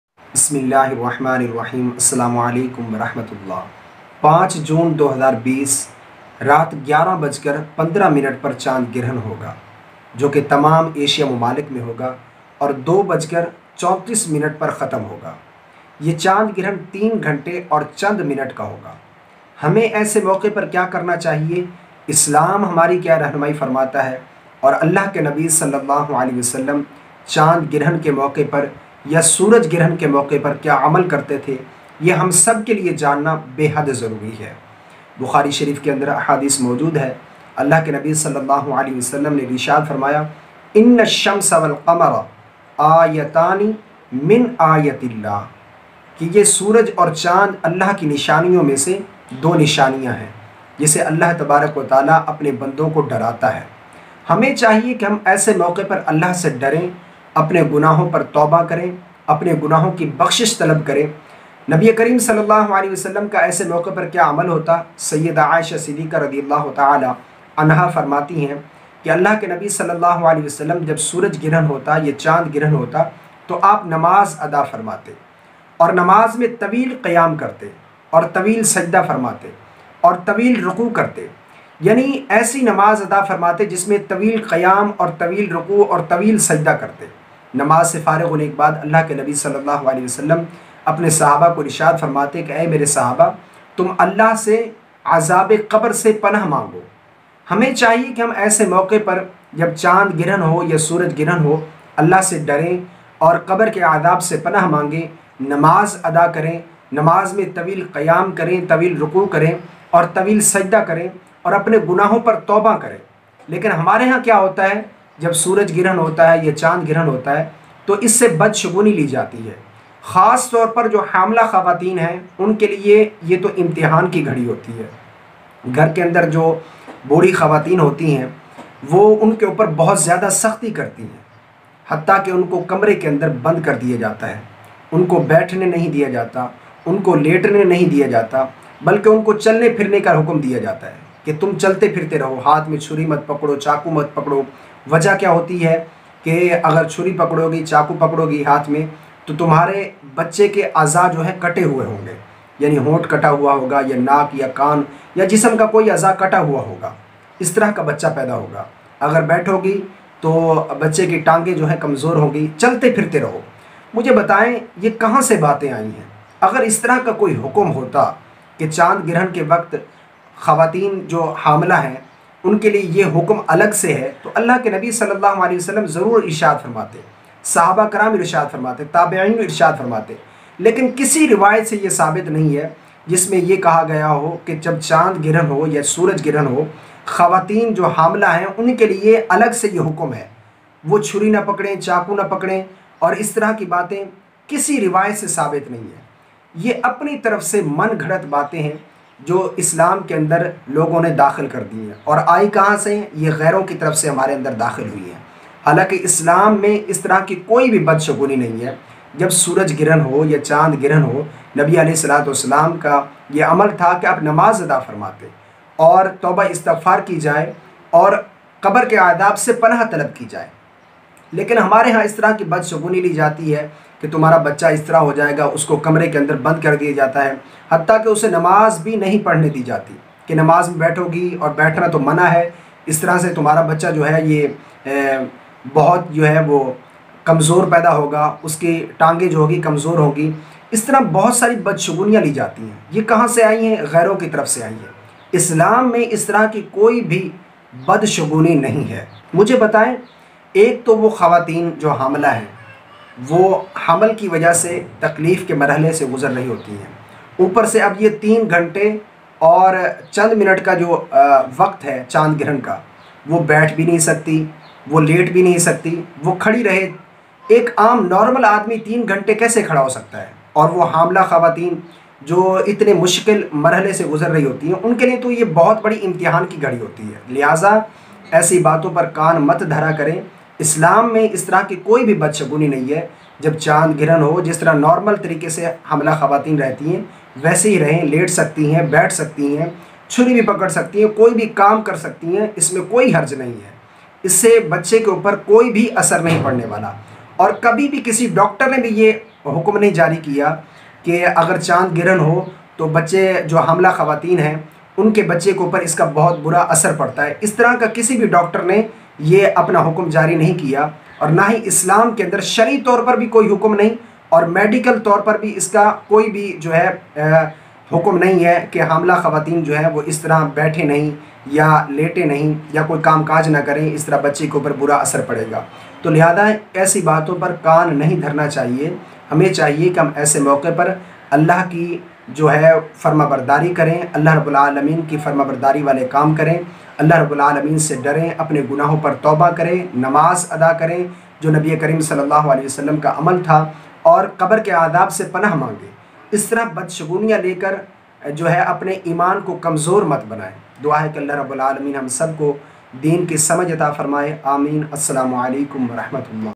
Hamla Aurat Chand Grahan Main Kiya Kare | Chandra Grahan Aur Pregnancy | Chand Grahan 05 June 2020
Hamla Aurat Chand Grahan Main Kiya Kare | Chandra Grahan Aur Pregnancy | Chand Grahan 05 June 2020 | NEA